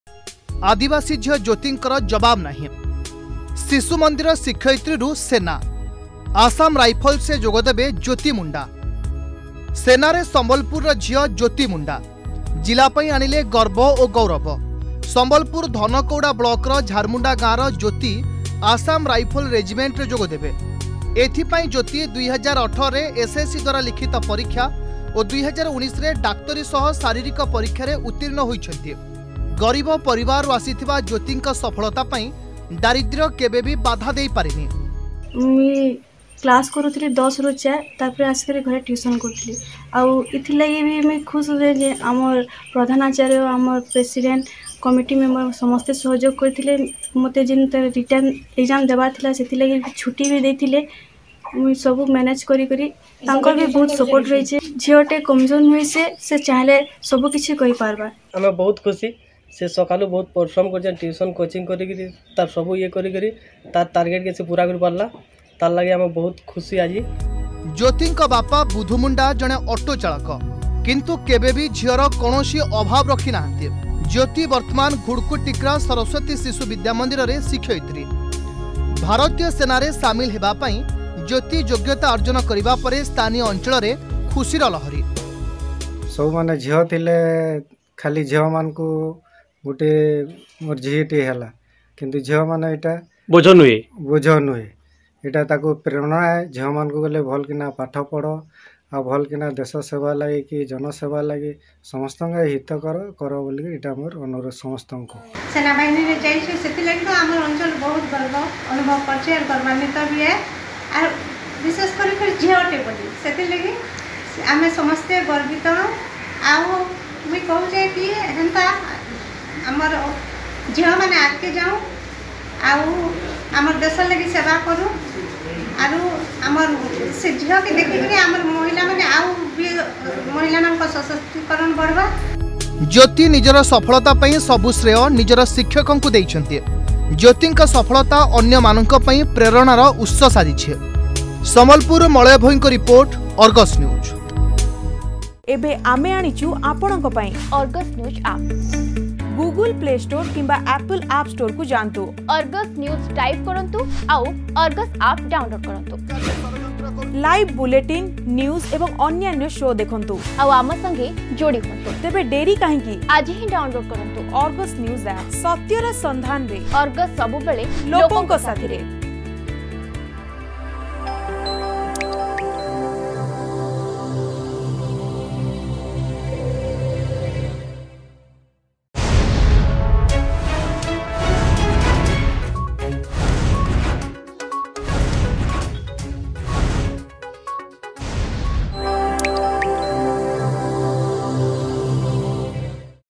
ଶିଶୁମନ୍ଦିର ଶିକ୍ଷୟିତ୍ରୀରୁ ସେନା
ସମ୍ବଲପୁର ଆଦବାସୀ ଝିଅ ଜ୍ୟୋତି ମୁଣ୍ତା ସାମିଲ ହେଵେ ଆସାମ ରାଇଫଲରେ । ବହୁ ବାଧାବିଘ୍ନ ପରେ ଶିଶୁମନ୍ଦିରରେ ଶିକ୍ଷୟତ୍ରୀ ହୋଇଥିଲେ । ଏବେ ପୁଣି ଆଉ ପାଦେ ଆଗକୁ ବଢି ସମ୍ବଲପୁର ଜିଲ୍ଲା ପାଇ ଗର୍ବ ଓ ଗୌରବ ଆଣିଛନ୍ତି । ସେପଟେ ଯେ ଏହି ଖବର ଜାଣିଵା ପରେ ଅଂଚଳରେ ଖୁସିର ଲହରି ଖେଲି ଯାଇଛି.